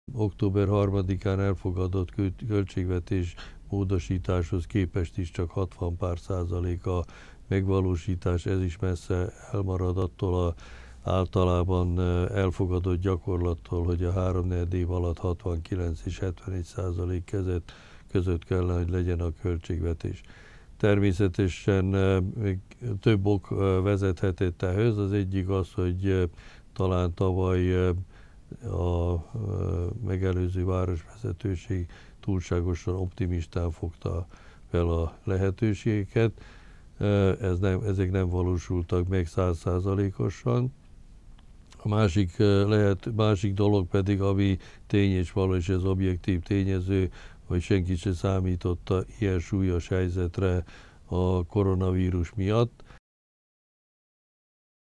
2020.11.09. - A fontos beruházásokat be akarják fejezni Szabadkán
Az alapvető cél, hogy a megkezdett beruházásokat befejezzük Szabadka város területén – fogalmazott Kern Imre.

Szabadka alpolgármestere a Szabadkai Magyar Rádió délelőtti műsorában elmondta: az egyik legfontosabb feladat, hogy befejezzék a Népszínház felújítását, illetve a palicsi wellnessközpont kiépítését. Ezek mellett jelenleg is folynak kisebb beruházások, mint amilyen a Szabadkai Gerontológiai Központ és a Kolevka gyermekotthon felújítási munkálatai. Kern Imre ugyanakkor hangsúlyozta, az új beruházások a következő év költségvetésétől függnek majd.